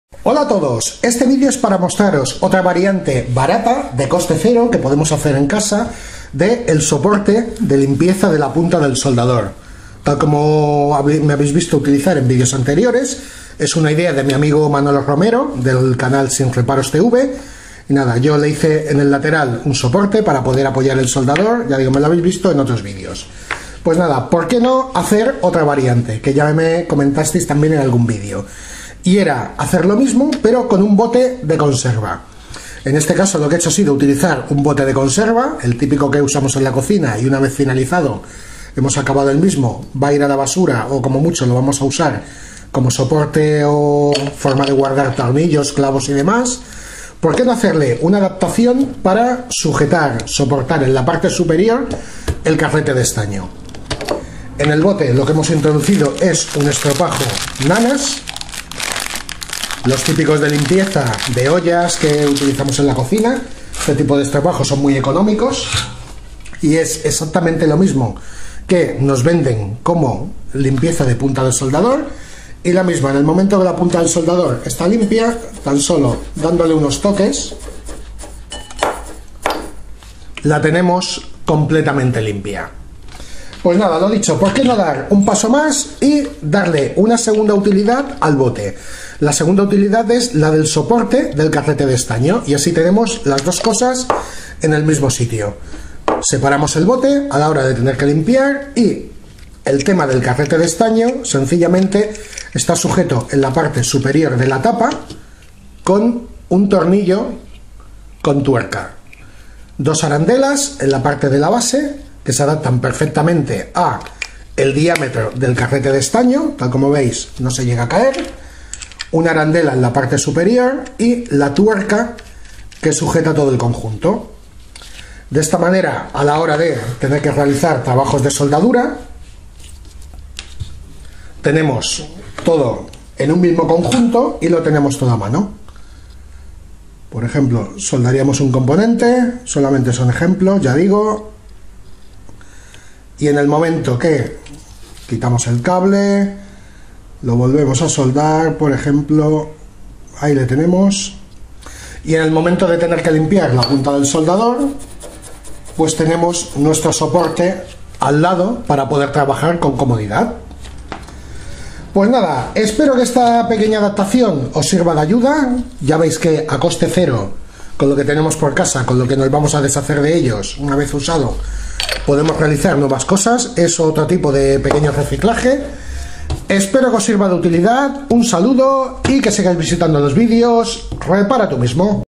COMO REALIZAR LIMPIAPUNTAS SOLDADOR, MAS SOPORTE ESTAÑO CASERO POR MENOS DE 1 €
Un sencillo limpiador de punta de soldador y soporte del carrete de estaño,   menos de 1 € y que se puede realizar reciclando la mayoría de materiales o incluso los podemos tener en casa.